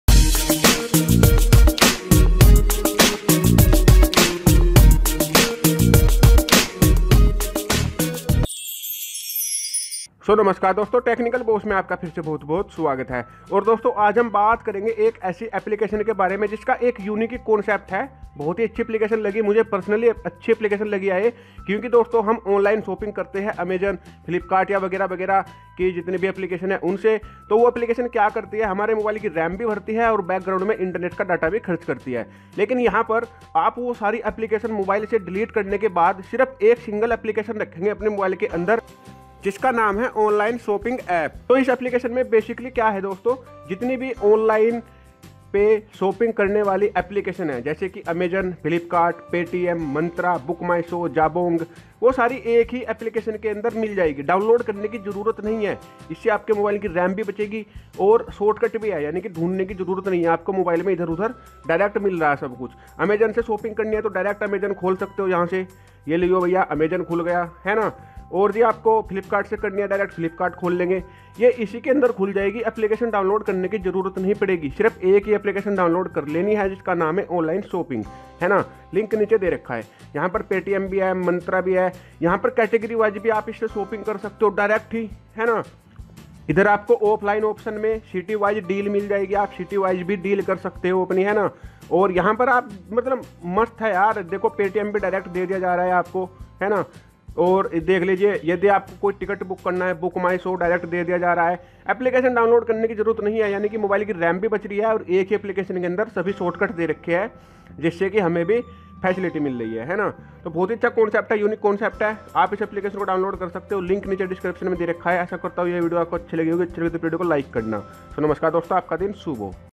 Android trick : 300+Apps in just 1App(2.5mB), Solve Android Storage and hanging Problem
Android tricks and tips to solve your storage and hanging issue by installing this one app. The android trick is by installing this one app, you can access 300+ apps inside it very smoothly and saves more memory on your android phone and helps to improve its speed and performance.

Thanks